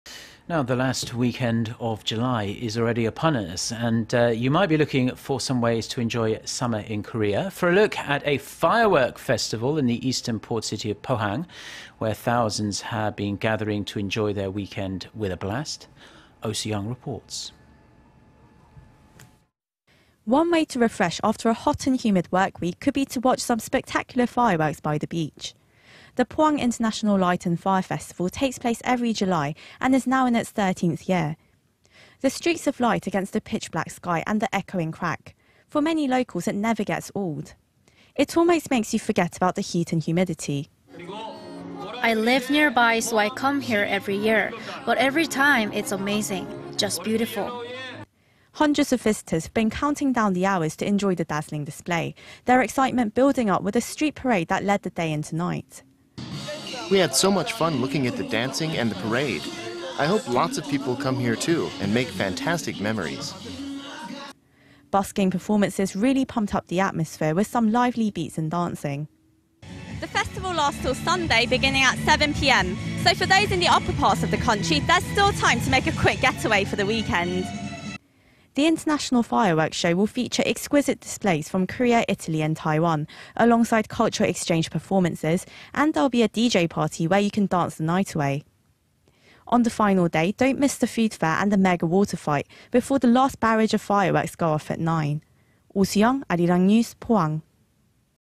Fireworks at Pohang International Light and Fire Festival
포항국제불빛축제
The last weekend of July is upon us... and you might be looking for ways to enjoy Summer in Korea.
For a look at a firework festival in the eastern port city of Pohang where thousands have been gathering to enjoy their weekend with a blast,... Oh Soo-young reports. One way to refresh after a hot and humid work week could be to watch some spectacular fireworks by the beach.
The Pohang International Light and Fire Festival takes place every July and is now in its 13th year.
The streaks of light against the pitch-black sky, then the echoing crack -- for many locals, it never gets old.
It almost makes you forget about the heat and humidity.

"I live nearby, so I come here every year. But every time it's amazing. Just beautiful."

Hundreds of visitors have been counting down the hours to enjoy the dazzling display,... their excitement building up with a street parade that led the day into night.

"We had so much fun looking at the dancing and the parade. I hope lots of people come here too and make fantastic memories."

Busking performances really pumped up the atmosphere with some lively beats and dancing.

The festival runs through Sunday and starts at 7PM. So for those in the upper parts of the country, there's still time to make a quick getaway.

The international fireworks show will feature exquisite displays from Korea, Italy and Taiwan,... alongside cultural-exchange performances AND there'll be a 'DJ party' where you can dance the night away.
On the final day, don't miss the food fair and the mega water fight before the last barrage of fireworks go off at nine.